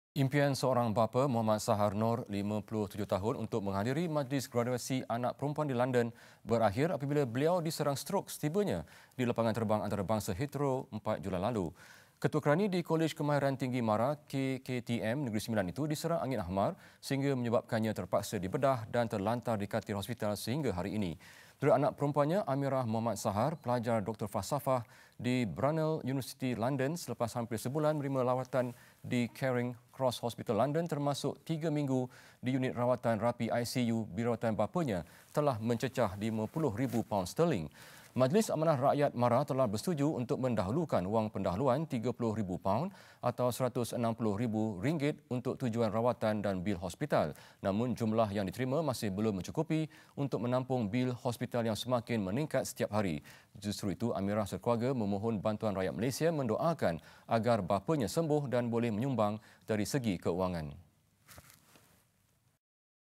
Bil Hospital Pegawai MARA Cecah Hampir RM200,000
Impian seorang bapa Mohamad Sahar Noor, 57 tahun untuk menghadiri majlis graduasi anak perempuan di London berakhir, apabila beliau diserang strok setibanya di Lapangan Terbang Antarabangsa Heathrow, 4 Julai lalu.

Ketua kerani di Kolej Kemahiran Tinggi Mara, KKTM Negeri Sembilan itu, diserang angin ahmar sehingga menyebabkannya terpaksa dibedah dan terlantar di katil hospital sehingga hari ini.

Menurut anak perempuannya Amirah Mohamad Sahar, pelajar Doktor Falsafah di Brunel University, London...

Selepas hampir sebulan menerima rawatan di Charing Cross Hospital, London termasuk, tiga minggu di unit rawatan rapi, ICU, bil rawatan bapanya telah mencecah 50,000 pound sterling.

Majlis Amanah Rakyat, MARA telah bersetuju untuk mendahulukan wang pendahuluan 30,000 pound atau RM160,000 untuk tujuan rawatan dan bil hospital.

Namun jumlah yang diterima masih belum mencukupi untuk menampung bil hospital yang semakin meningkat setiap hari.

Justeru itu, Amirah sekeluarga memohon bantuan rakyat Malaysia mendoakan agar bapanya sembuh dan boleh menyumbang dari segi kewangan.